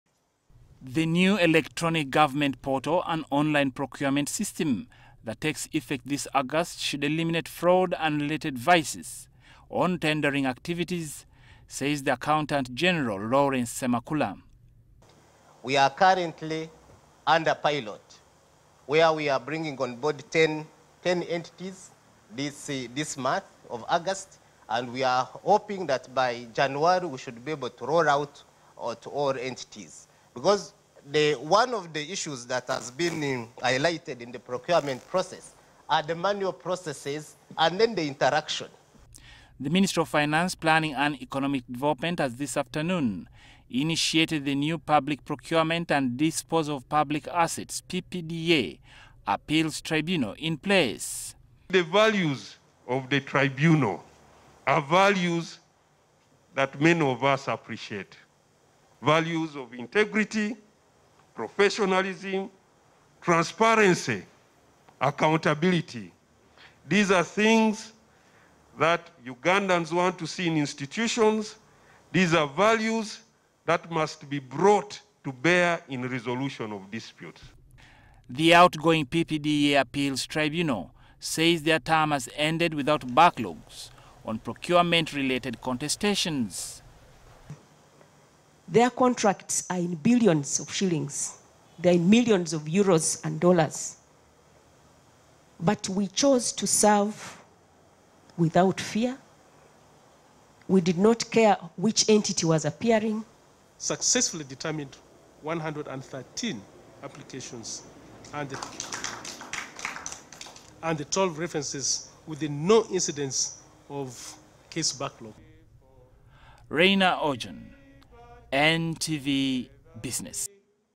PUBLIC PROCUREMENT: Appeals tribunal inaugurated
After many promises, government now says the E-Procurement system will come into force this month. Accountant General Lawrence Semakula, says that 10 entities will take part in the pilot including Ministry of Finance,  UNRA, KCCA and PPDA among others. He was speaking during the induction of the new PPDA Appeals Tribunal, at the Ministry of Finance in Kampala.